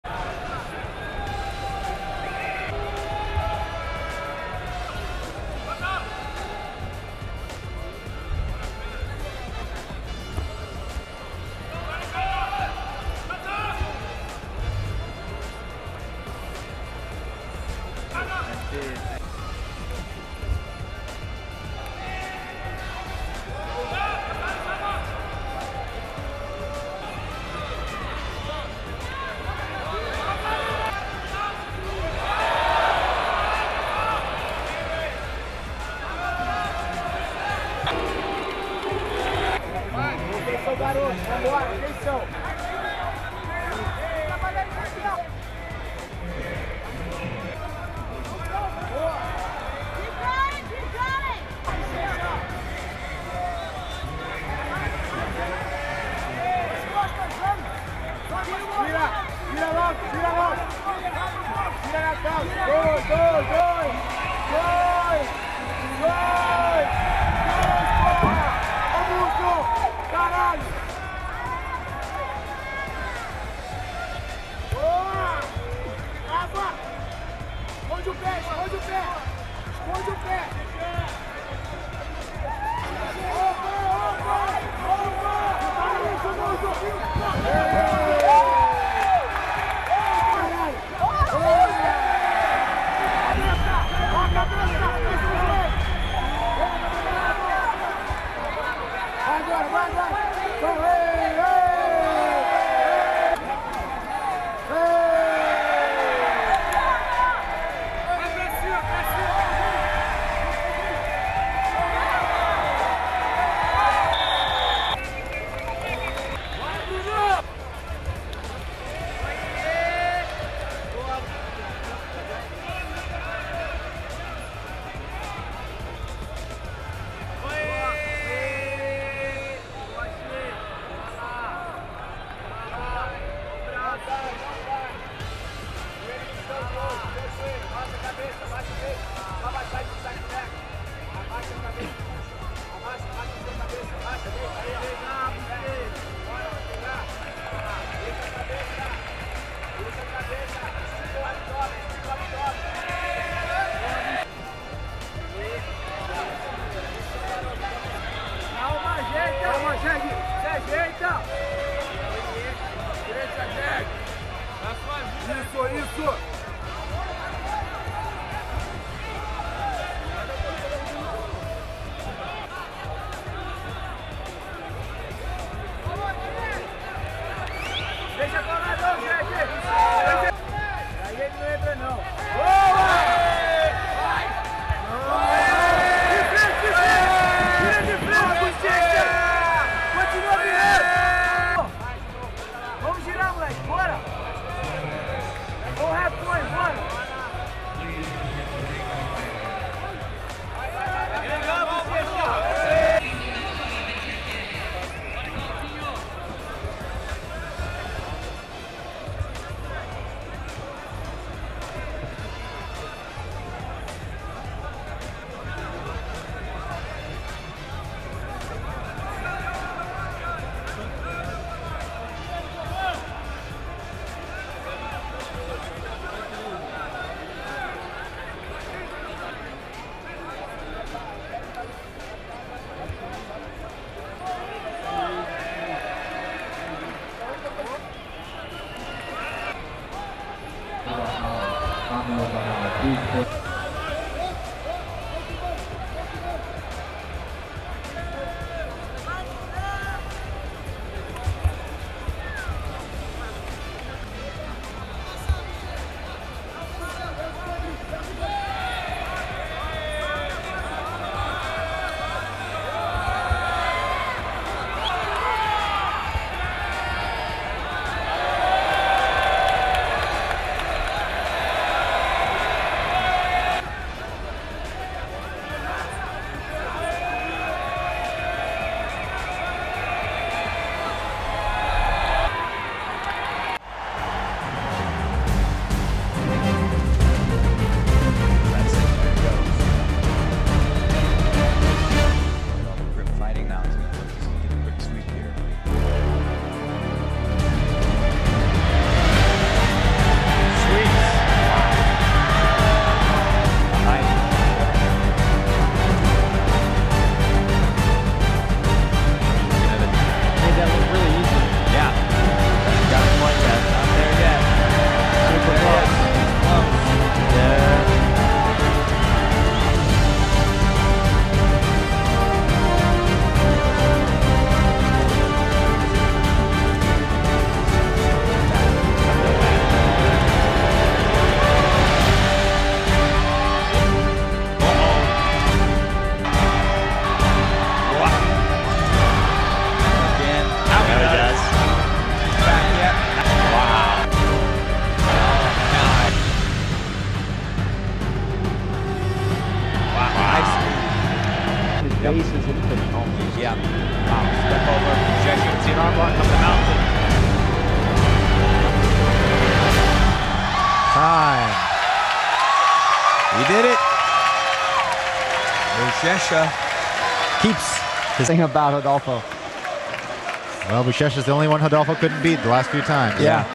Buchecha Worlds 2013 Highlights
🇺🇸I remember right after winning the 2012 Worlds Weight and Absolute divisions. During the title celebration, my friend Chaves, who is practically my older brother, told me this:
“Bro, next year you’ll have to win again, this way people won’t say you were lucky.”
That was his way of congratulating me lol. But in a way it helped me a lot because it was with that mentality that I went into the 2013 Worlds and managed to win Weight and Absolute again, my 3rd and 4th World Title as a Black Belt!

🇧🇷Lembro que logo após ganhar o mundial peso e absoluto de 2012, durante a comemoração do Título, meu amigo Chaves que é praticamente meu irmão mais velho me falou a seguinte frase:
“É Bichao, pra vagabundo não falar que foi sorte, ano que vem tu vai ter que ganhar peso e absoluto de novo!”
Essa foi a forma dele de me dar parabéns rs. Mas de certa forma me ajudou muito pq foi com essa mentalidade que eu fui para o Mundial de 2013 e consegui novamente conquistar Peso e Absoluto. Meu 3 e 4 Título Mundial na Faixa Preta!